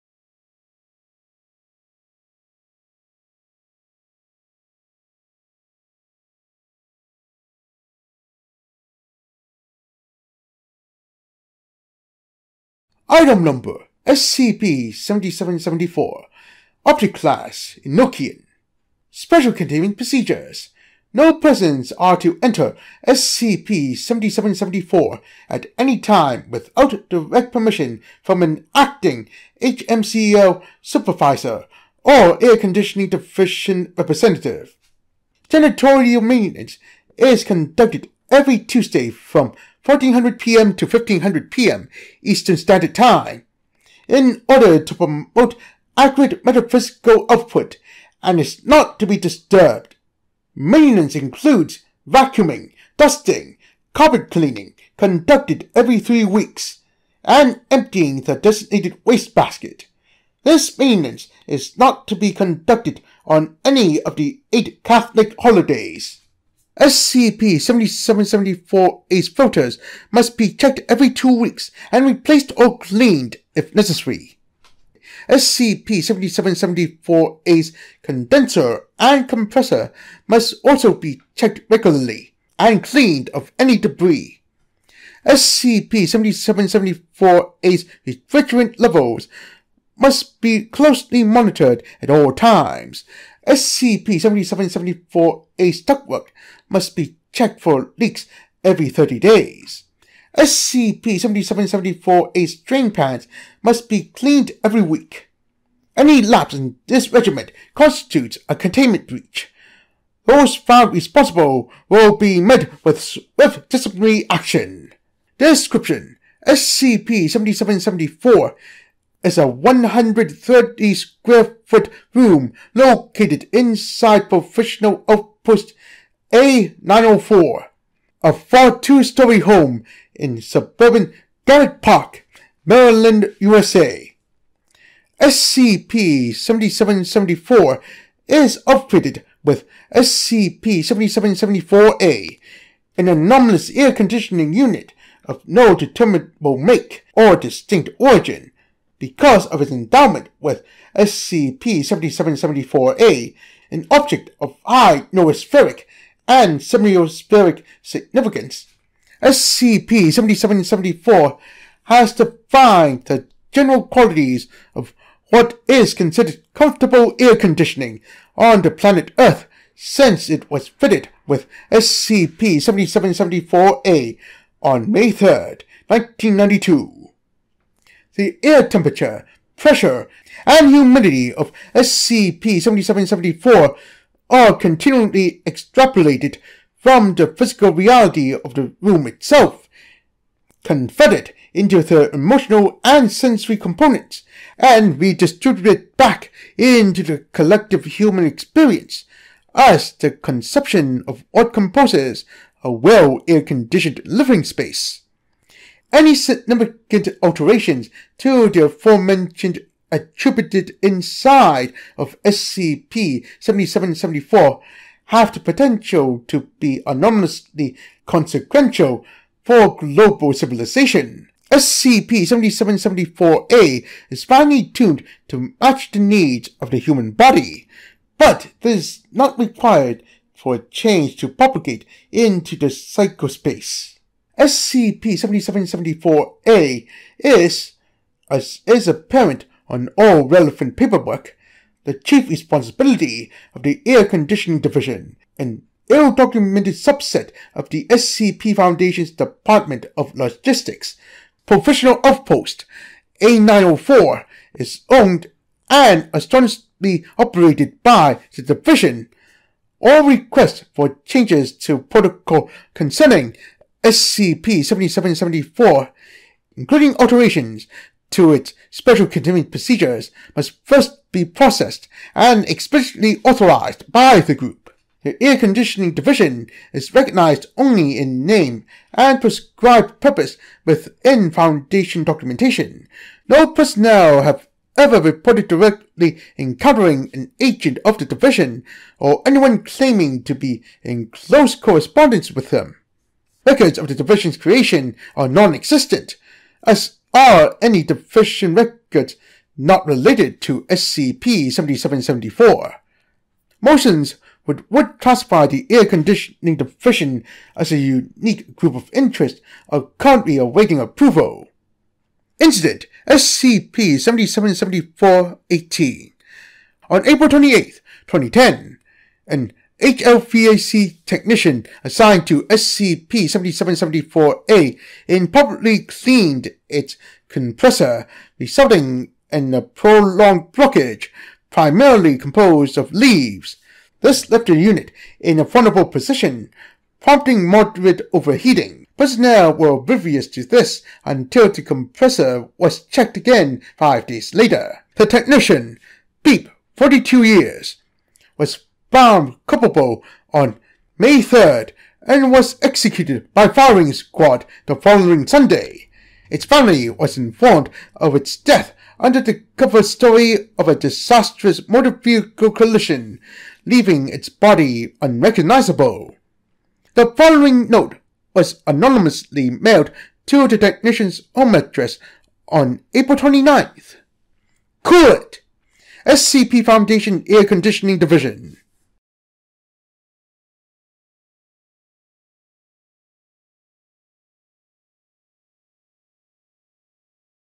SCP 7774 Room Temperature Room| SCP Maintained by the Department of Air Conditioning
SCP-7774 is a room in Provisional Outpost-A904, which is a faux two-story home, that is said to be the best air-conditioned room in the world.